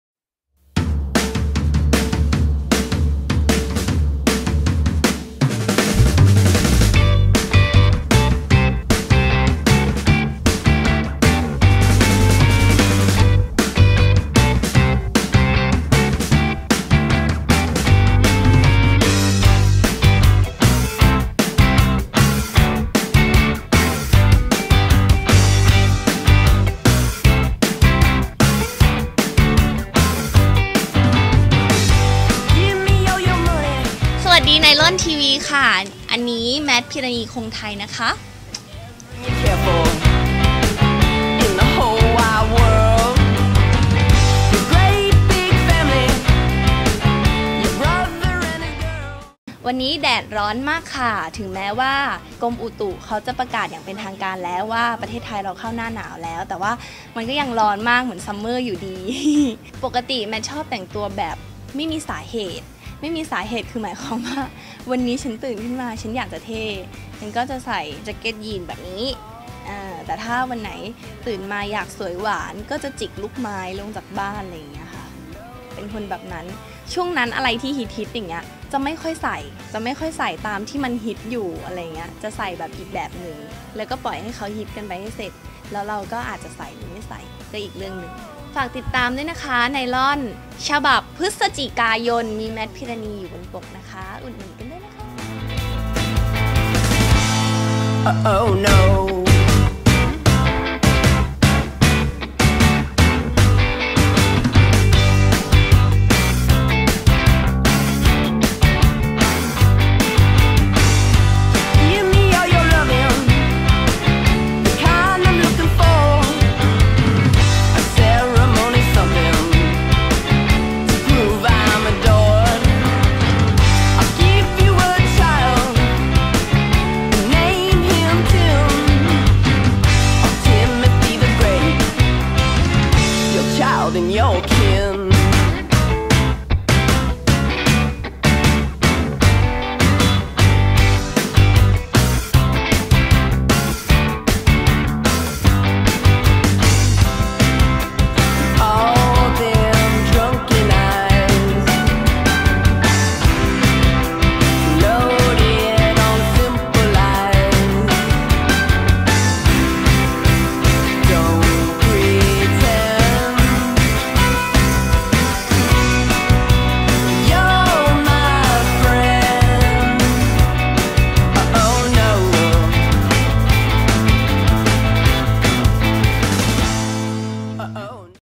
NYLON TV + MATT PEERANEE : The Travel Issue
พร้อมเดินทางไปกับเธอคนนี้รึยัง แมท-ภีรนีย์ คงไทย เนื่องจากเป็นคนกลัวความผิดหวัง ก็เลยไม่ค่อยตั้งเป้าหรือคาดหวังอะไรมาก มองแค่วันนี้กับพรุ้งนี้แล้วทำให้ดีที่สุด